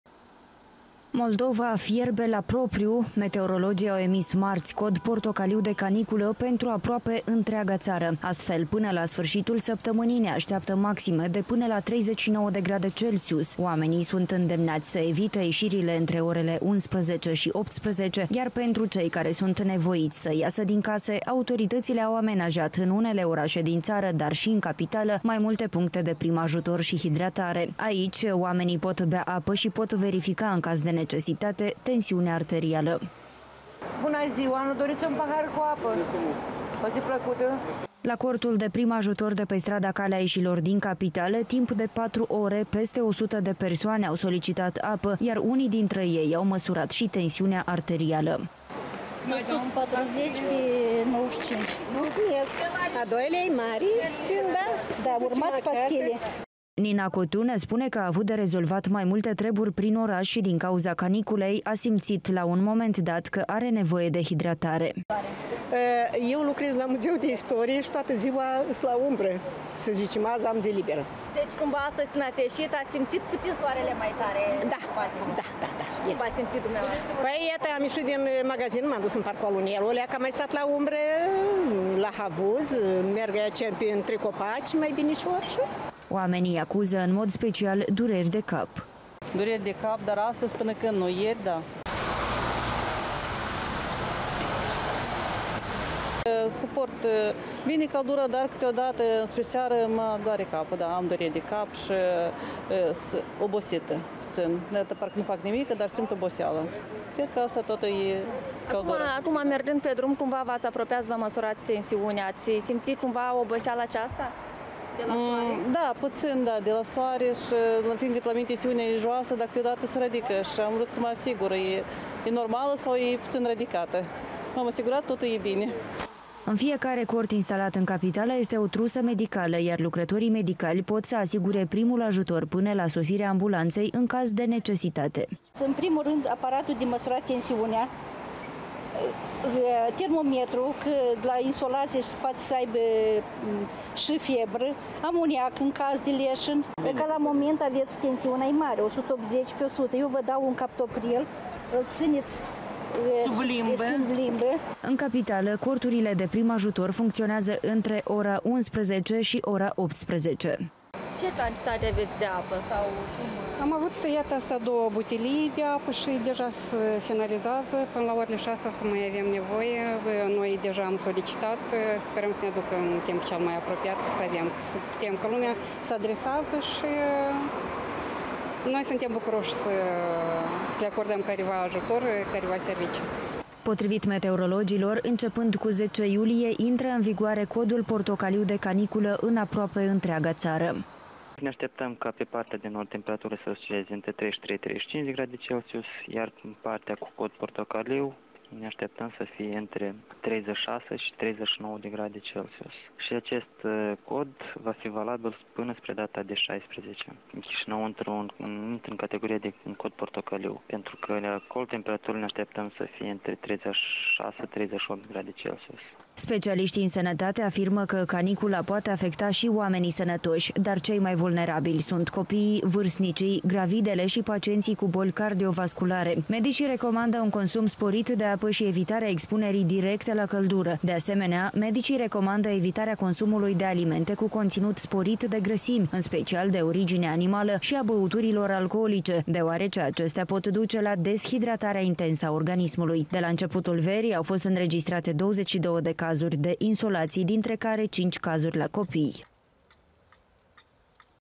INFO ONE - COD PORTOCALIU DE CANICULĂ PENTRU APROAPE ÎNTREAGA ȚARA
Moldova fierbe la propriu. Meteorologii au emis marți Cod portocaliu de caniculă pentru aproape întreaga țara. Astfel până la sfârșitul săptămânii ne așteaptă maxime de până la 39 de grade Celsius.  Oamenii sunt îndemnați să evită ieșirile între orele 11 și 18, iar pentru cei care sunt nevoiți să iasă din case autoritățile au amenajat în unele orașe din țară dar și în Capitală mai multe puncte de prim ajutor și hidratare. Aici oamenii pot bea apă și pot verifica în caz de necesitate tensiunea arterial.